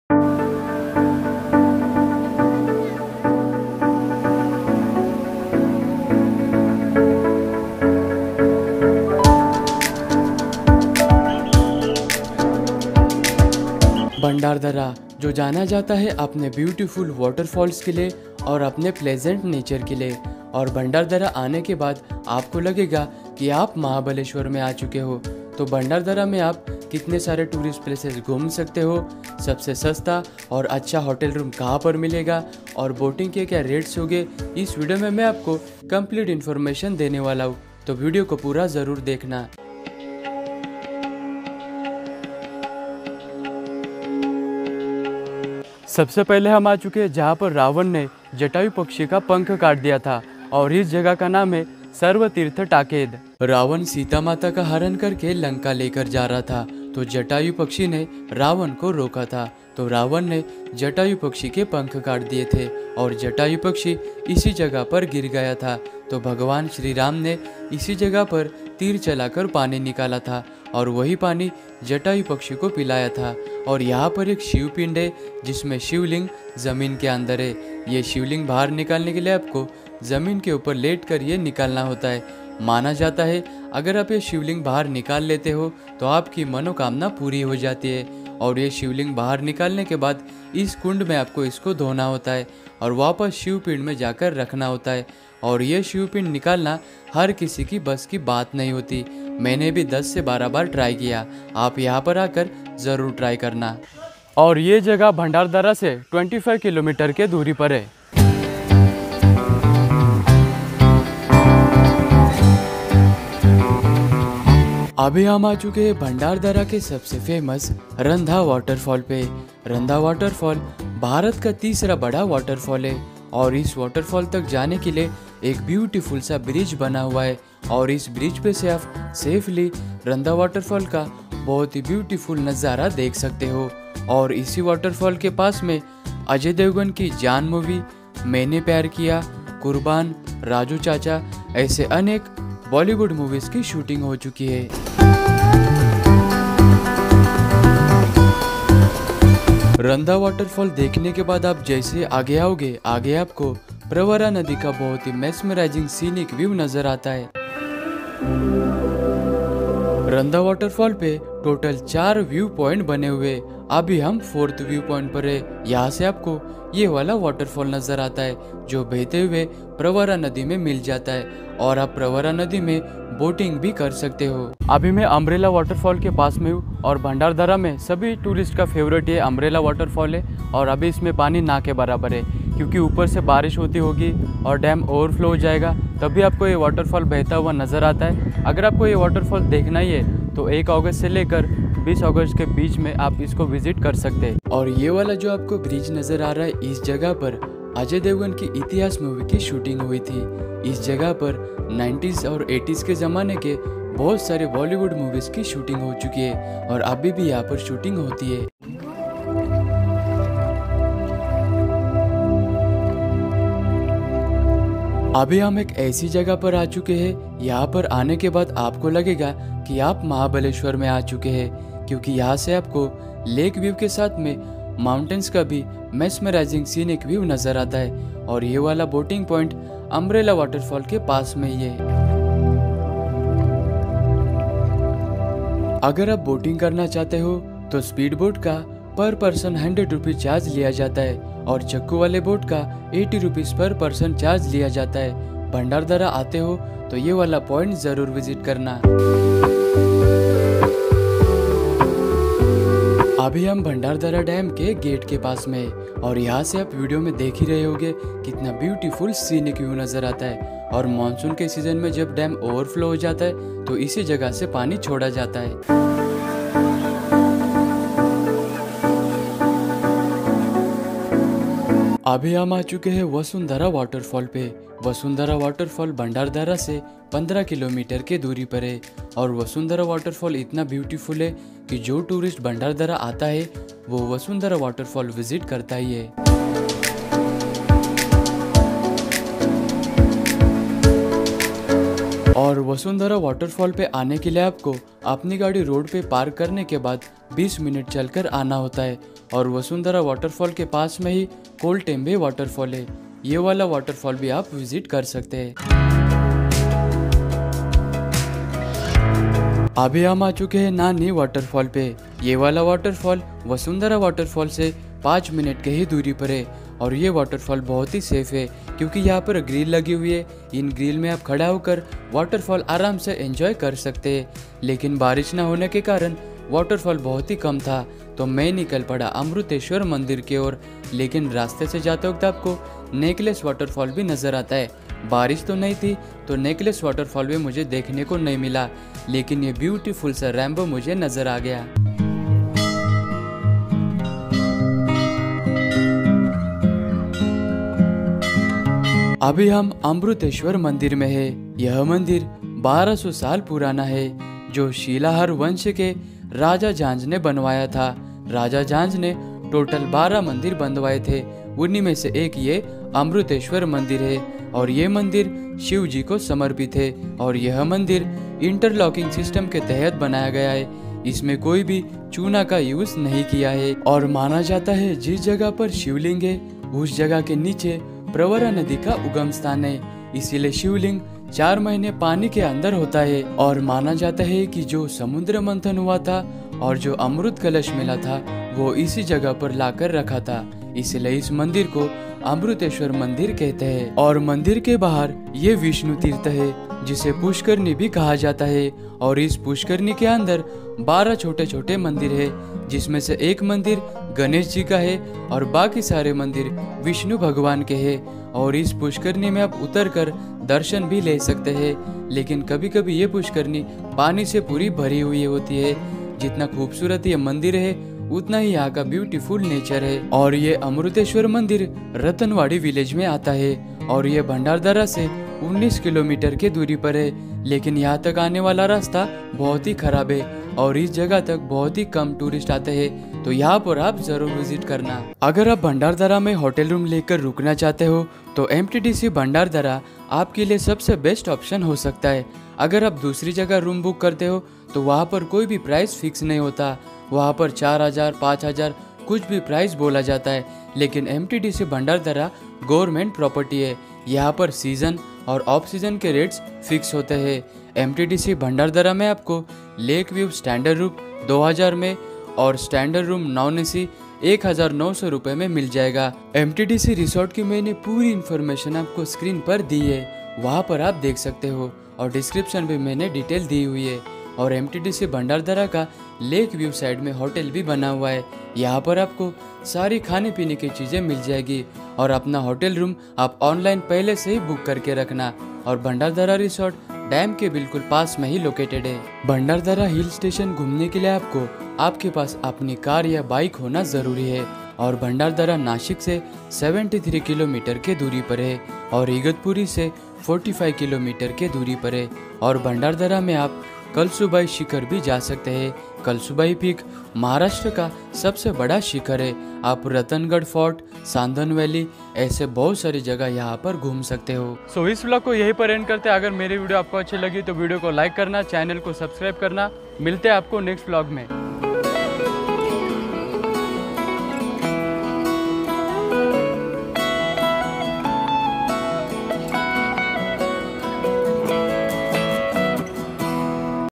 Bhandardara Hill Station In Monsoon | Bhandardara Tourist Places | Hotels Waterfall Boating भंडारदरा
भंडारदरा हिल स्टेशन | Bhandardara Tourism
 In This Bhandardara Vlog I Gives You Complete Information About Bhandardara Hill Station Places To Visit In Bhandardara Hotels & Resorts Bhandardara Tourist Points Bhandardara Boating

 Bhandardara Is 165 KM From Mumbai 73 KM From Nashik 161 KM From Pune & 45 KM From Igatpuri For Bhandardara Nearest Railway Station Is  Igatpuri Railway Station And Nashik Road Railway Station

Places To Visit In Bhandardara
1. umbrella waterfall
2. randha waterfall
3. bhandardara dam
4. bhandardara boating
5. vasundhara waterfall
6. koltembhe waterfall
7. nanhe ghat waterfall
8. necklace waterfall
9. amruteshwar temple
10. ratangad fort trek
11. sandhan valley
12. kalsubai trek
13. Igatpuri hill station
14. Arthur Lake